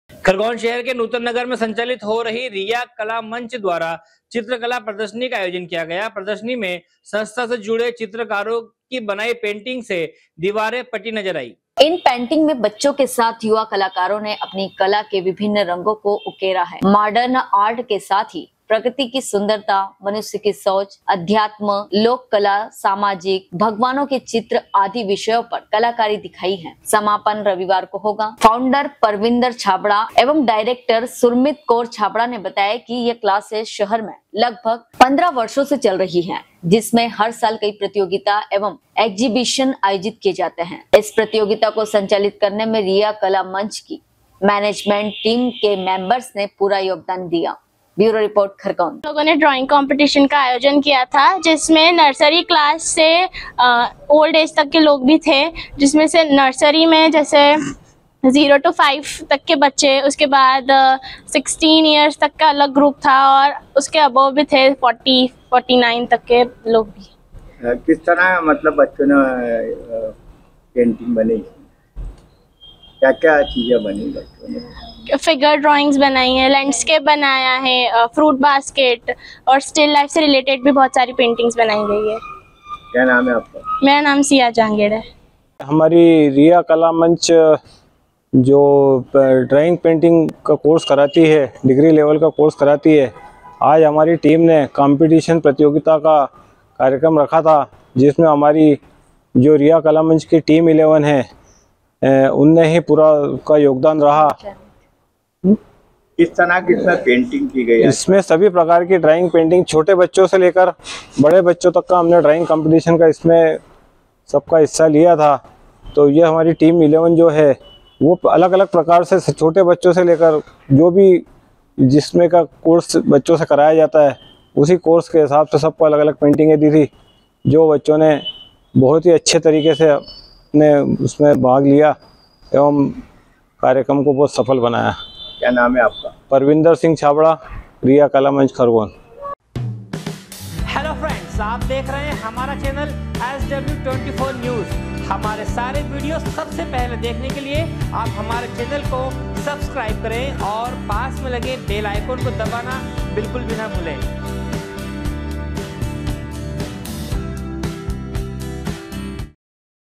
शहर के नूतन नगर में संचालित हो रही रिया कला मंच द्वारा चित्रकला प्रदर्शनी का आयोजन किया।
शहर के नूतन नगर में संचालित हो रही रिया कला मंच द्वारा चित्रकला प्रदर्शनी का आयोजन किया। प्रदर्शनी में संस्था से जुड़े चित्रकारों कि बनाई पेंटिंग से दीवारे पटी नजर आई।